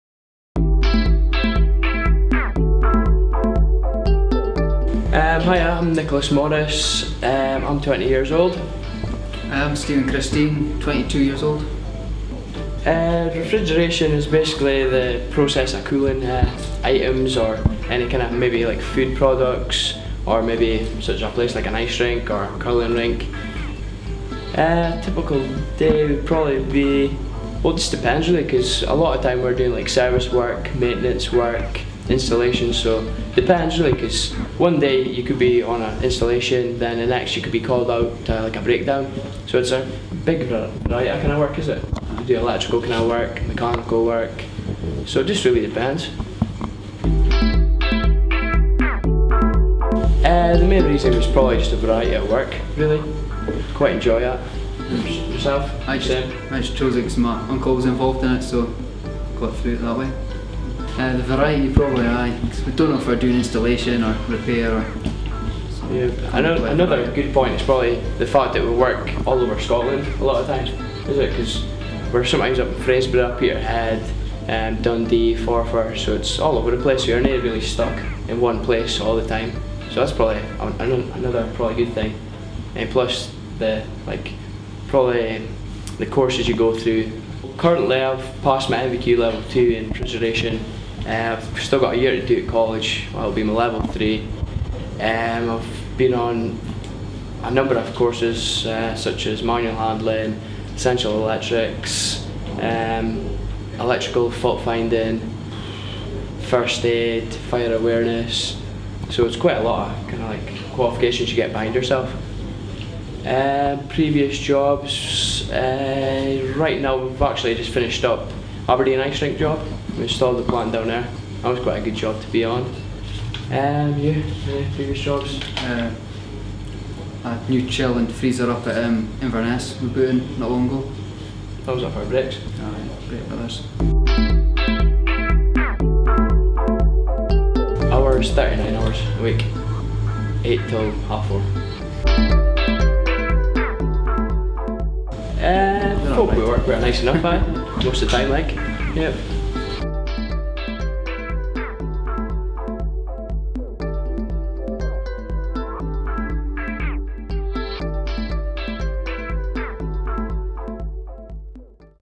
Nicholas Morris and Stephen Christine, Refrigeration Engineers
Nicholas Morris and Stephen Christine talk about their work as Refrigeration Engineers in Aberdeen